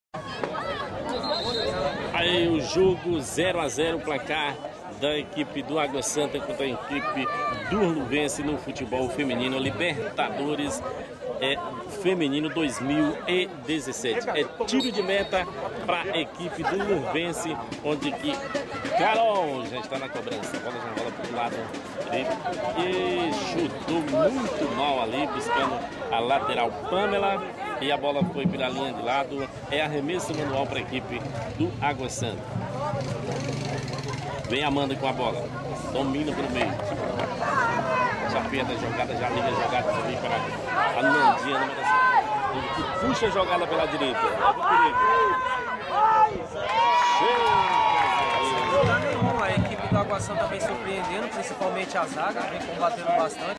Copa Libertadores Feminino - Agua Santa 1 x 0 Luvence - parte 2 - STI Esporte
Copa Libertadores Feminino - Agua Santa(Diadema) 1 x 0 Luvence(Embu Guaçu) - parte 2 - Jogo de Ida
Estadio Distrital de Vila Alice   em 01/10/2017
narração: Bira
Imagens:Jota Alves
STI - Sistema de Transmissão Integrada